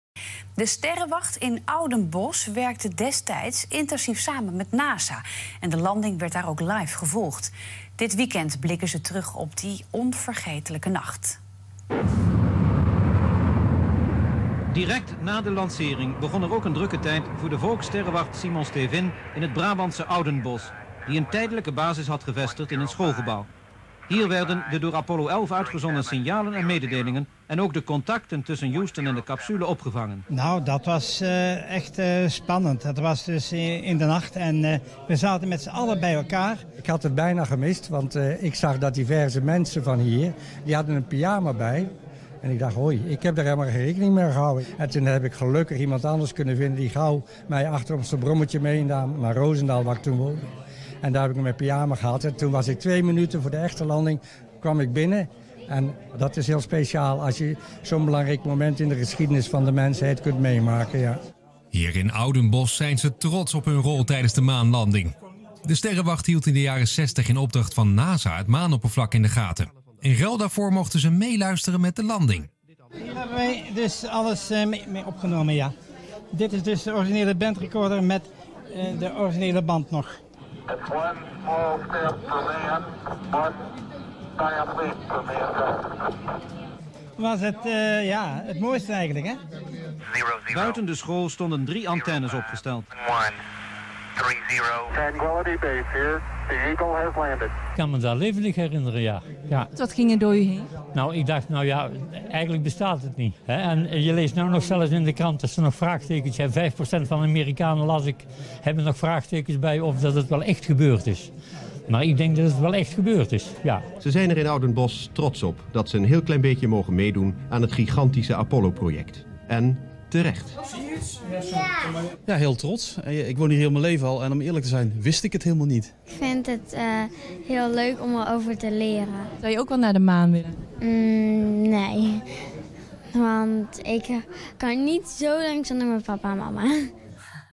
Sterrenwacht Tivoli Extra
Op 20 juli 1969 zette Neil Armstrong als eerste mens voet op de maan. Om dit feit luister bij te zetten organiseert Sterrenwacht Tivoli een MAANifestatie op 20 en 21 juli 2019.